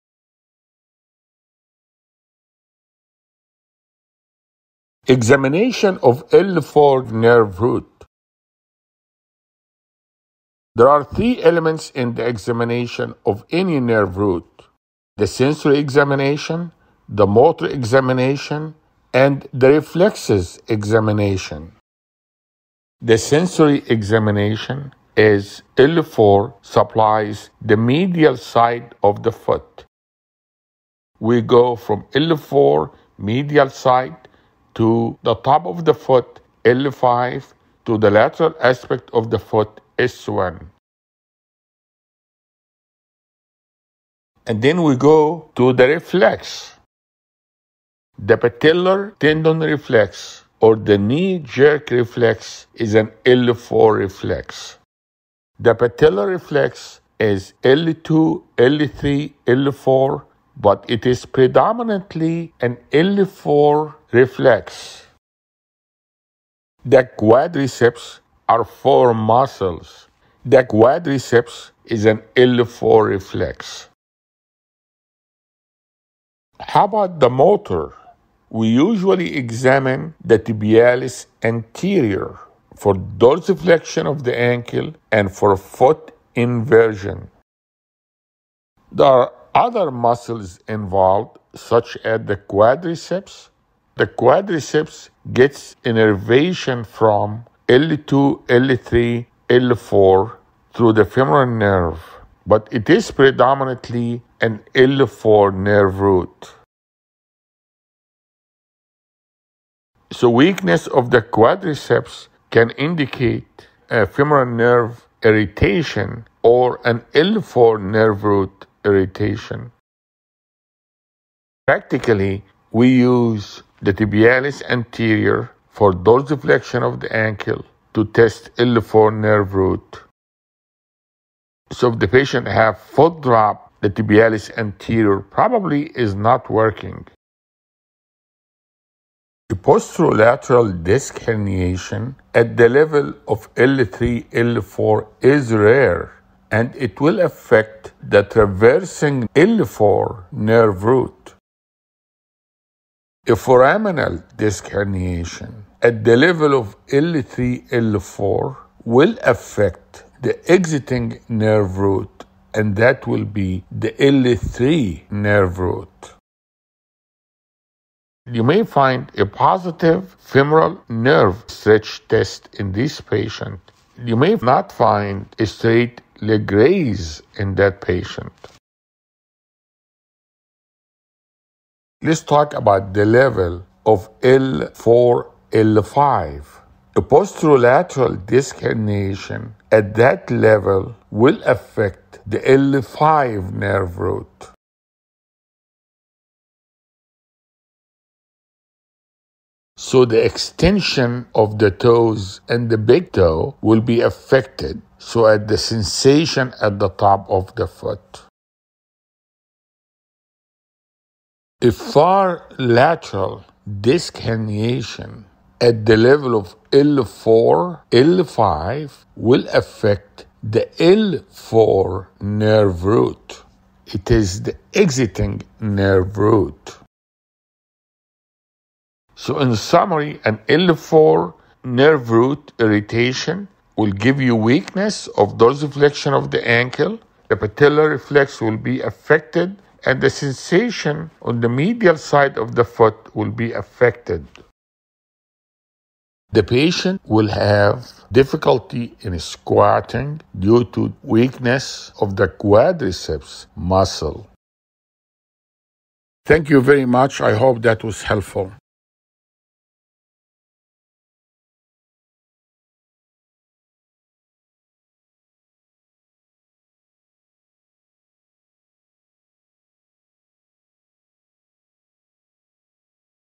Examination Of L4 Nerve Root - Everything You Need To Know - Dr. Nabil Ebraheim
Dr. Ebraheim’s educational animated video describes examination of the L4 nerve root.